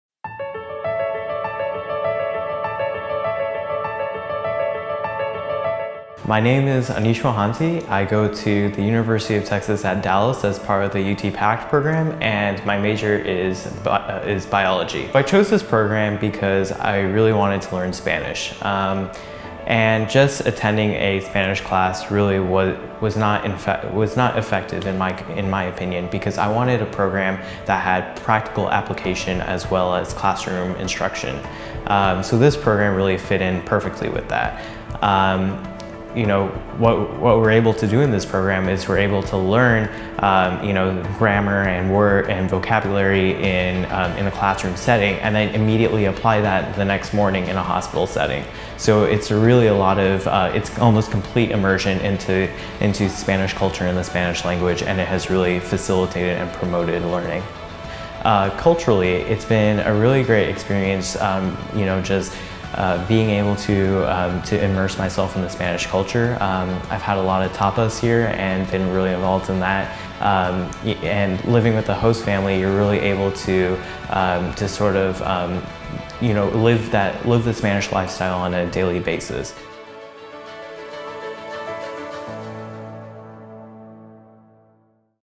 I knew NO Spanish before I went to Seville
Ahneesh received EMT training and shadowed doctors and nurses in Seville, Spain while receiving Spanish language training.